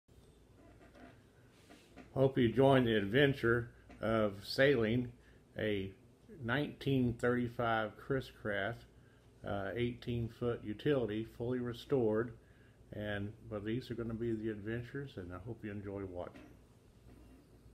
Adventures of the Alice K fully restored 1935 Chris Craft 18' Utility
Boating in beautiful Corpus Christi TX. Coastal Water..in a fully restored
."Antique 1935 Chris Craft 18' Utility" .
.Back in the day Corpus Christi was touted as ..
."The Naples of the Gulf" .
..Starting this adventure, having fun, seeking beauty at every turn ,..
...while slipping along, traveling through the travels of life .
. .Just a little more by boat..!! .tony